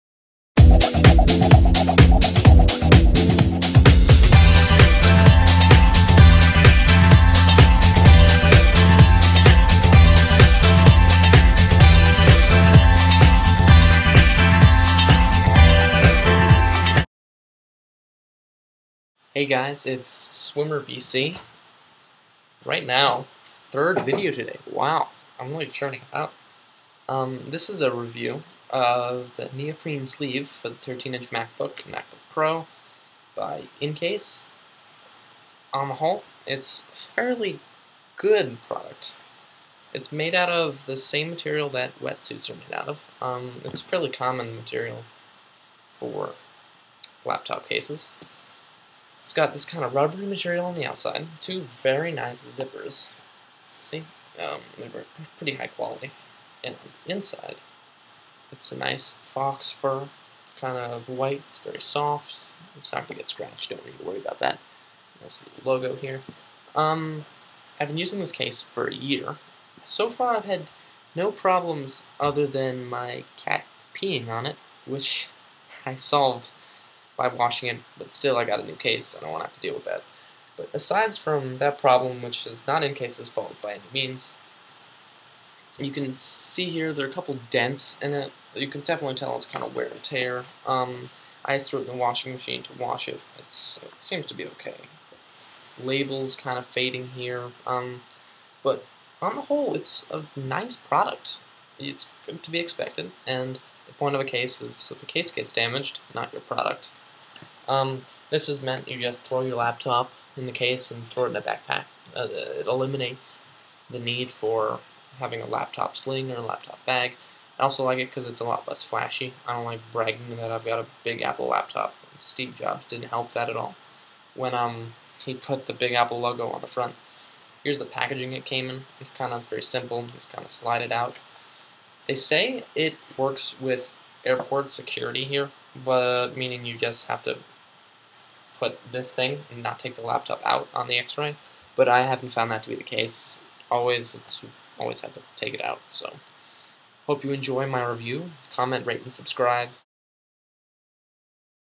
Incase Laptop Sleeve Review
This isn't the usual bargain technology that I do, i know...
Cost - $39.99 on Apple's website
It works really well. I've been using it heavily (so has my cat), so there is some wear and tear on it. It does it's job, no complaints.